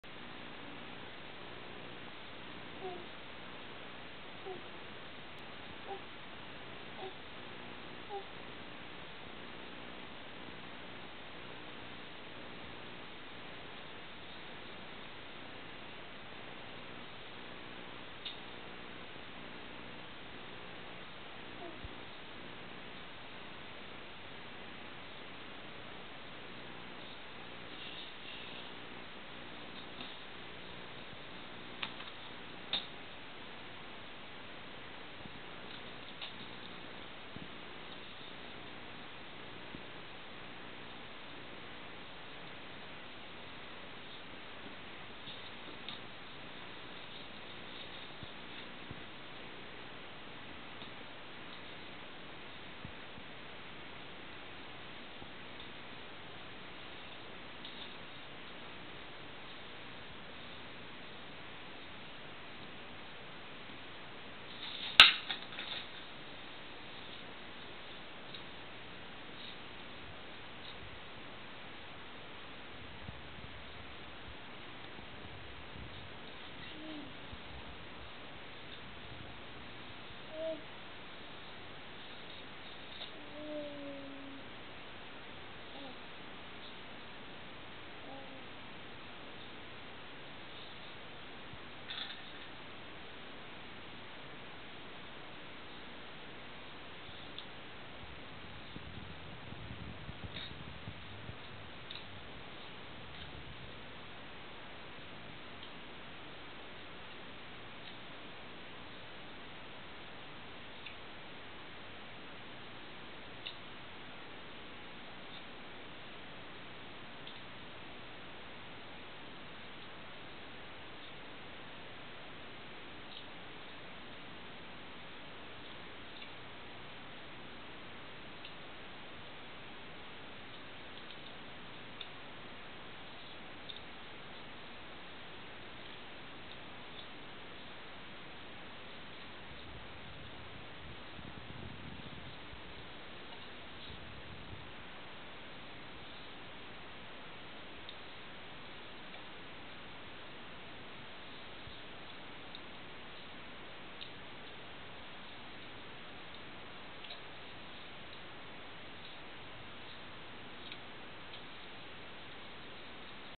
August 22, 2009
Mikee reading a book.